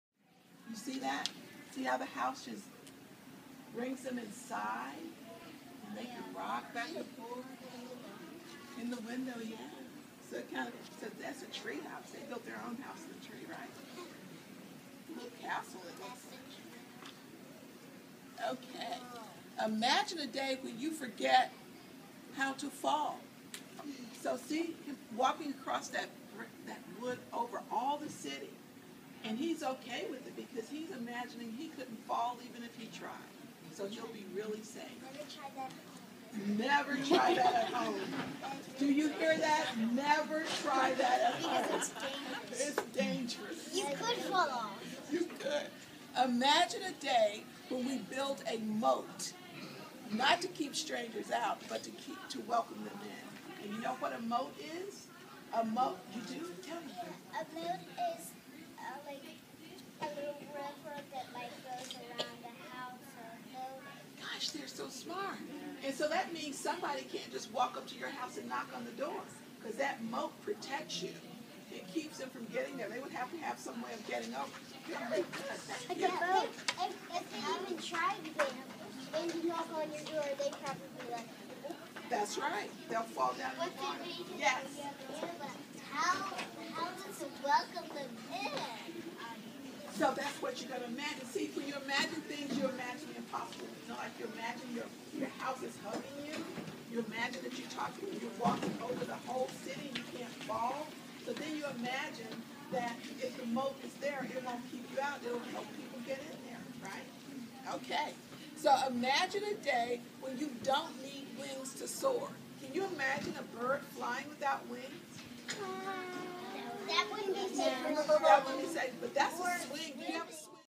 041713 Grantsville Elementary
CTN Video - Dr. Lillian M. Lowery, Maryland State superintendent reading to the students in recognition of national Reading Month.